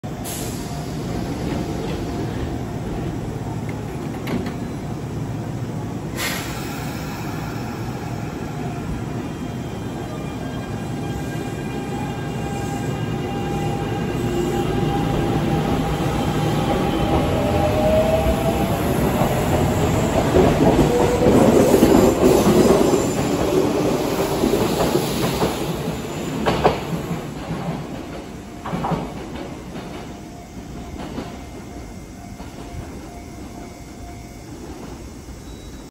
【切り離されて化け運用】近鉄6400系（Mi01）＋6419系（Mi19）富田林行き 発車
17時29分発

撮影：古市駅（2024/08/05）